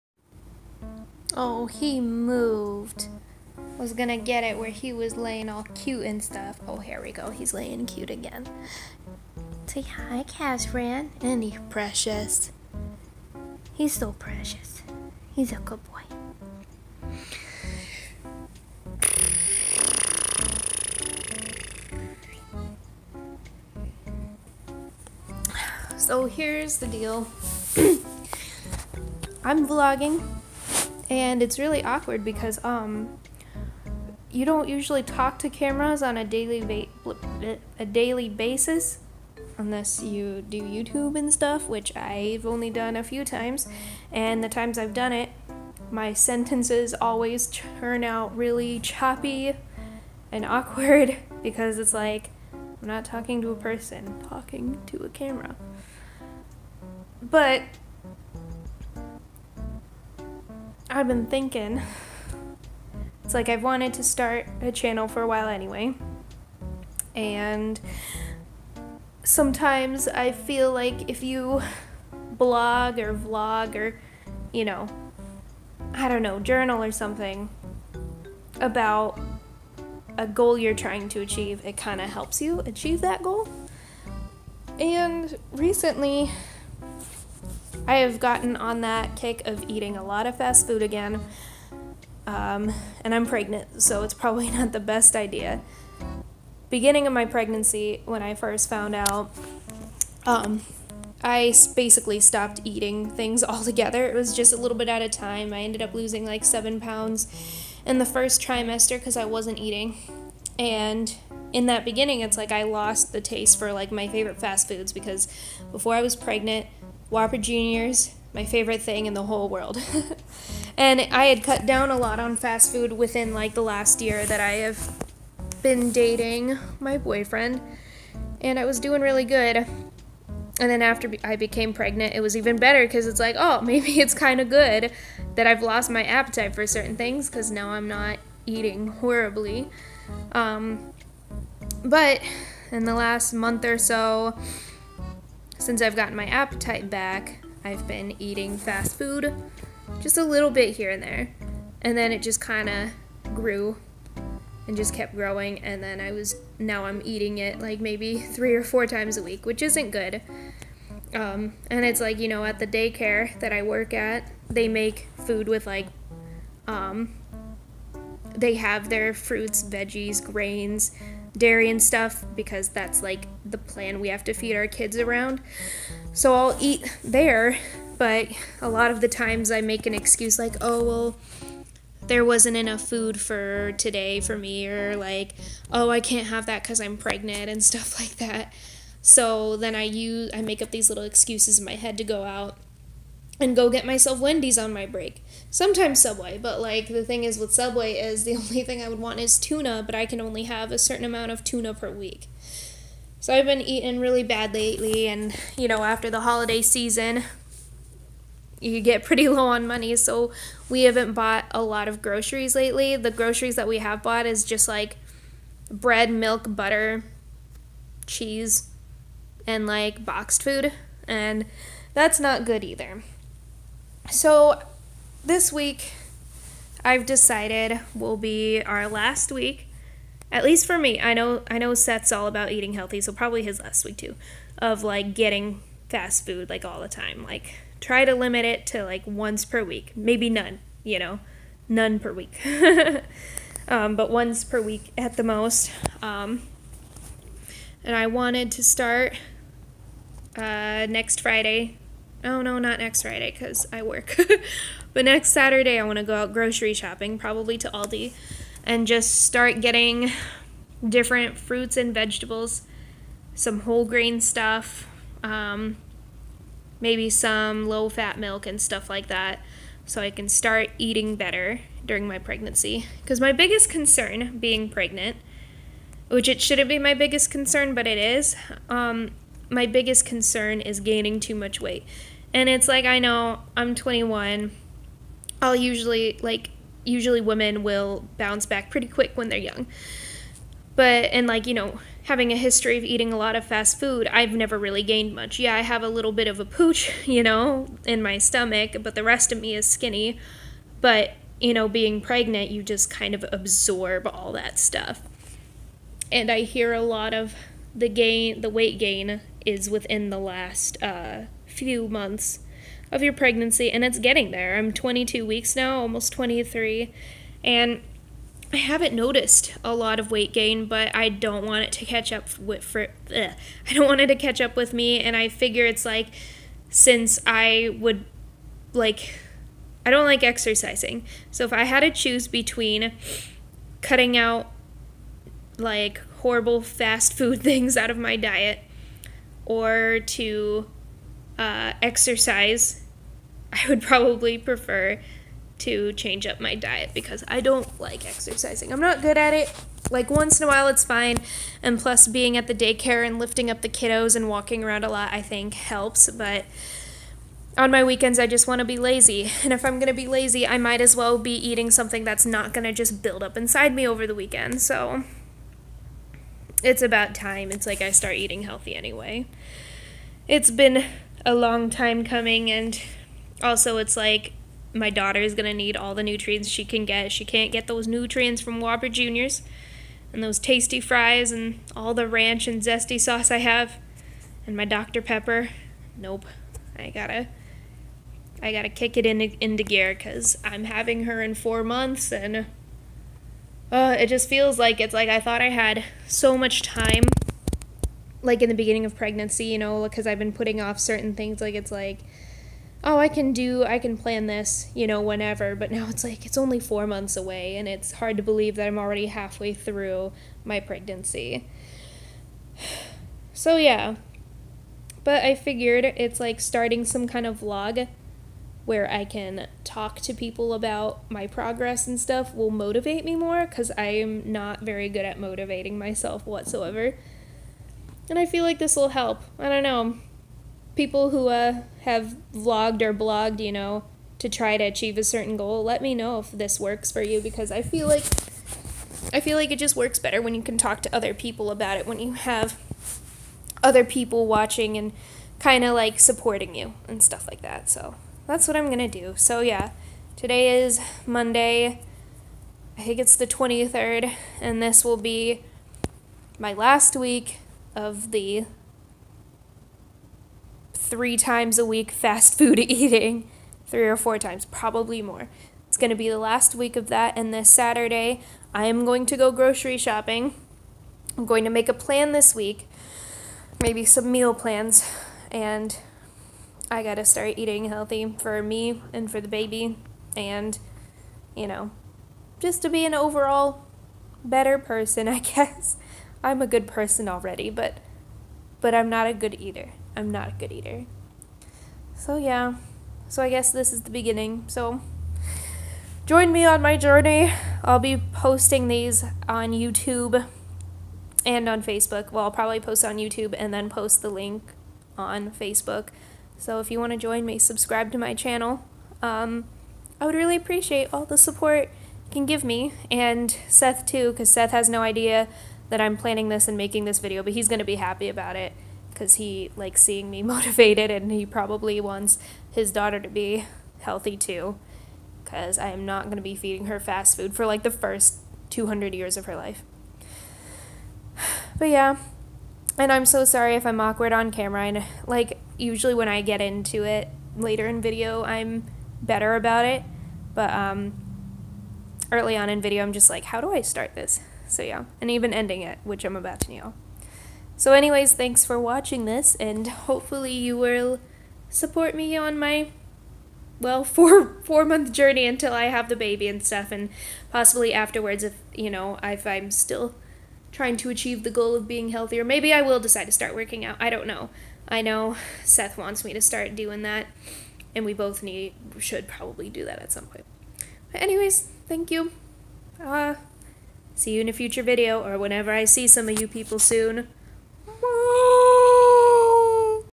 Vlog 1-- Starting Fresh!
WELCOME to my first official vlog!
Sorry I am awkward on camera! That will get better as I film more!
In this video, I talk about my bad eating habits and how I'm going to overcome them, for the sake of my pregnancy and well-being!
I am almost 23 weeks pregnant! ❤ Sorry I started vlogging it later in the game, hopefully all of you will still join me in these last few months of my pregnancy and beyond!